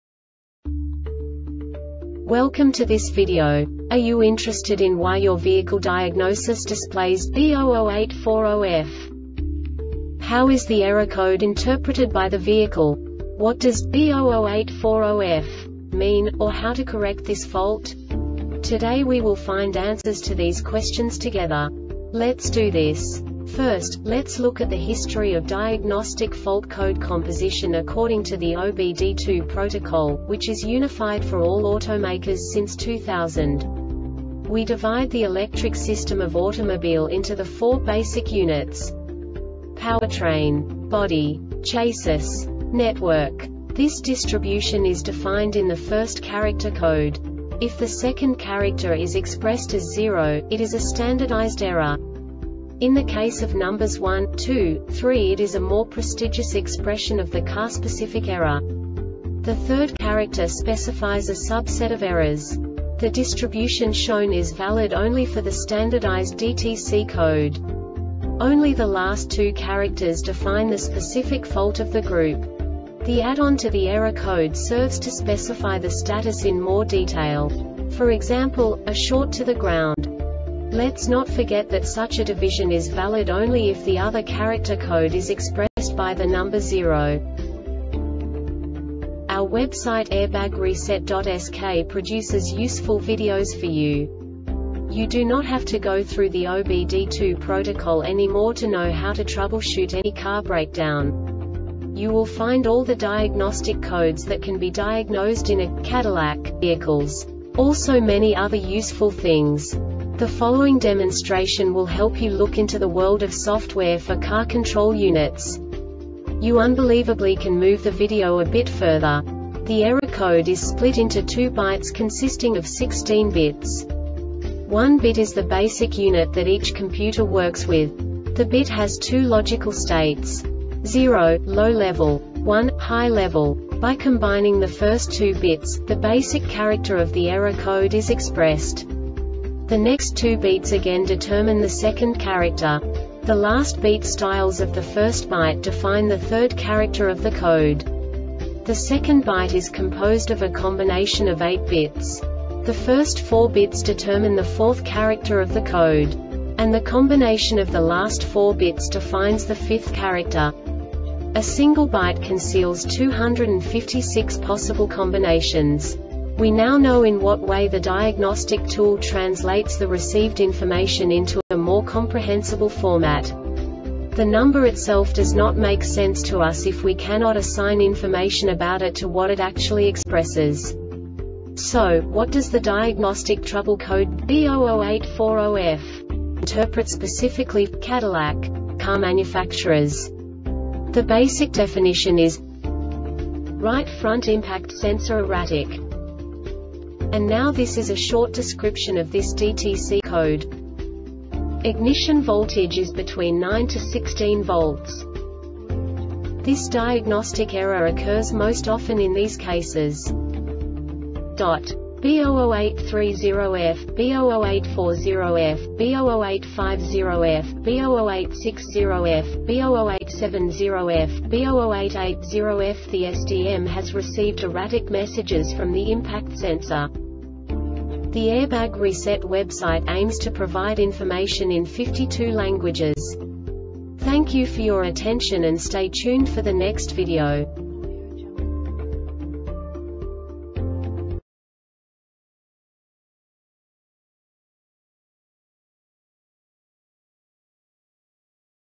DTC Cadillac B0084-0F Short Explanation
Contents:
0:21 Basic DTC analysis according to OBD2 protocol standard.
1:48 Insight into programming
2:58 A diagnostically understandable description of the Diagnostic Trouble Code
3:05 Basic error definition